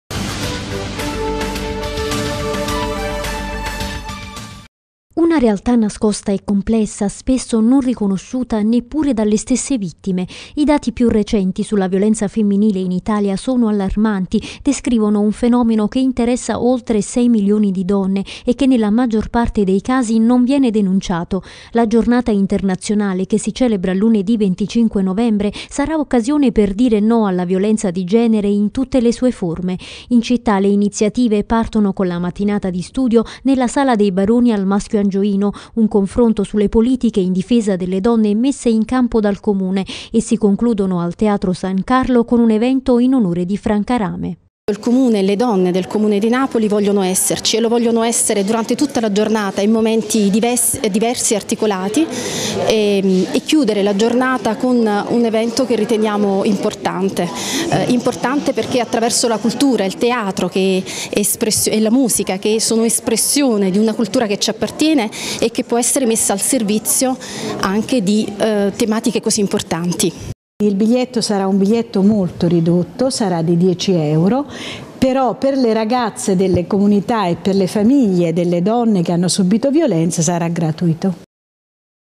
25 novembre, contro la violenza sulle donne
Anche la città di Napoli celebra la ricorrenza con una giornata di iniziative che si concluderanno al Teatro San Carlo con uno spettacolo dedicato a Franca Rame.